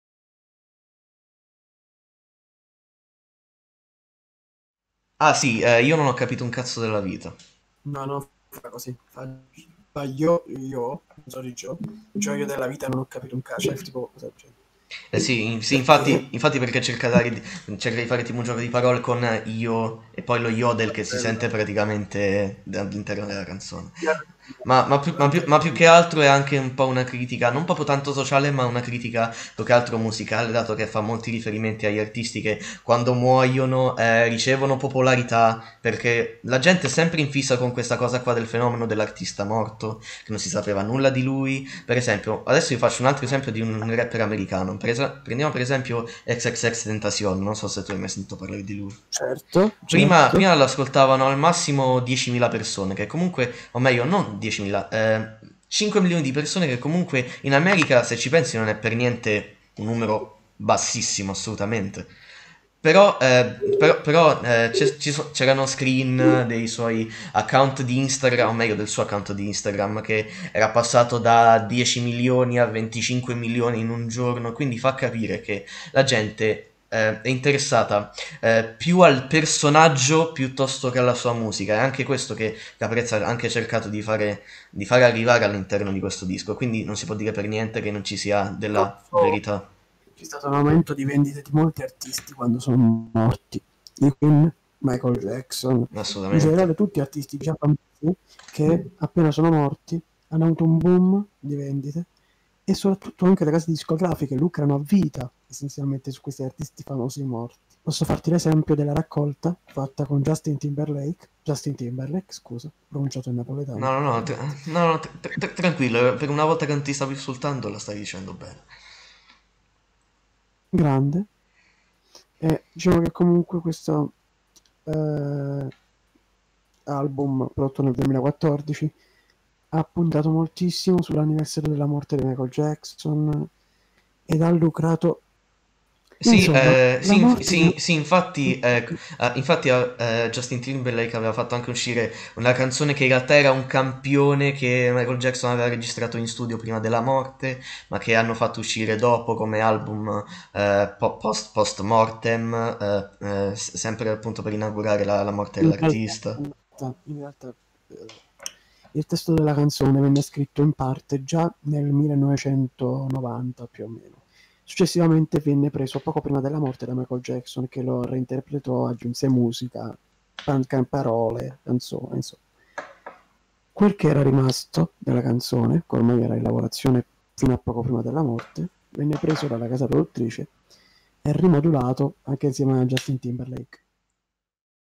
IL SUCCESSO DOPO LA MORTE (World Of Music & ByAngel Tube) - Copertina made by
Molti artisti hanno avuto un *boom* incredibile di vendite dopo il loro spegnimento musicale.
In questo video cercheremo di dare la nostra opinione sull'argomento, prendendo qualche esempio di artisti famosi!